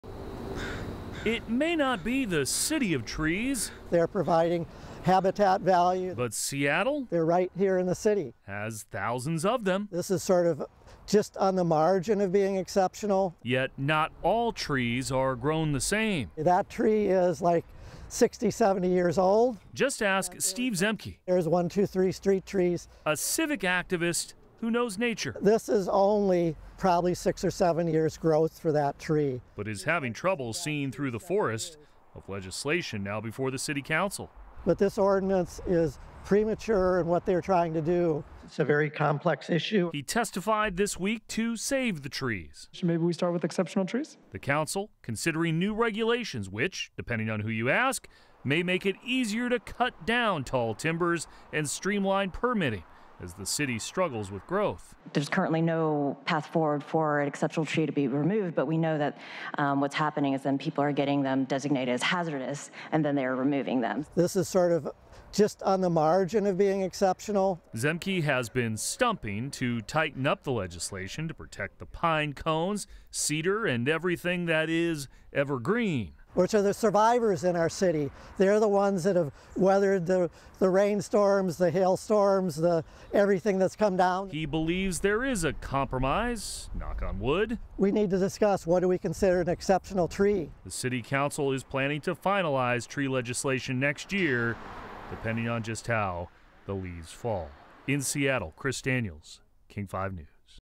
New Seattle laws could chop down tree population
The City of Seattle is talking about drastically changing tree laws. It could mean changes for even the tallest timbers - and not everyone is happy about it.